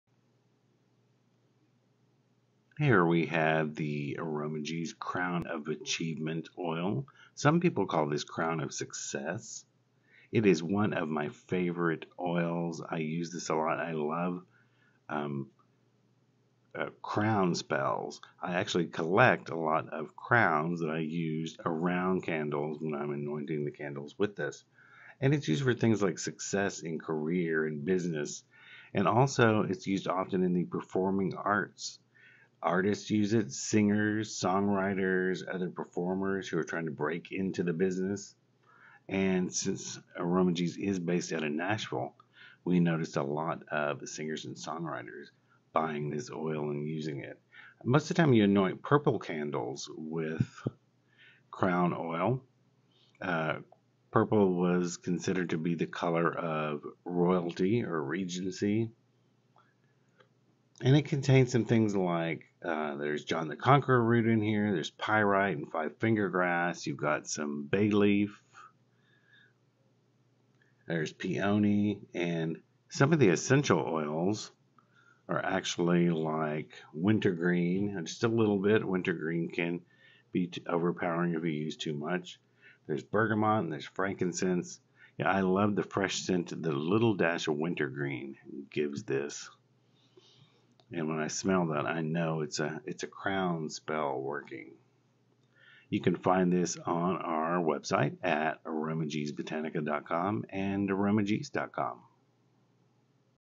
Crown of Achievement and Success hoodoo oil - spell for career, business, and performing arts.
Crown of Achievement hoodoo oil is used for success in career, business, and the performing arts. Especially good for singers, song writers, and other performers trying to break into the business. Based out of Nashville, I have a few clients who are singers and songwriters. The songwriters tell me they take the Crown oil and rub it on their pen/pencil while composing. Crown oil can be used to achieve success in anything you are attempting or working towards such as: a promotion, writing a paper or a book, in your arts and crafts, to help reach weight loss goals - anything that you want to succeed in.

Crown of Achievement oil contains John the Conqueror root, pyrite, five finger grass, bay leaf, peony, and essential oils of wintergreen, bergamot, frankincense, and other herbs and essential oils. Use purple candles for this oil. Purple was considered a royal color.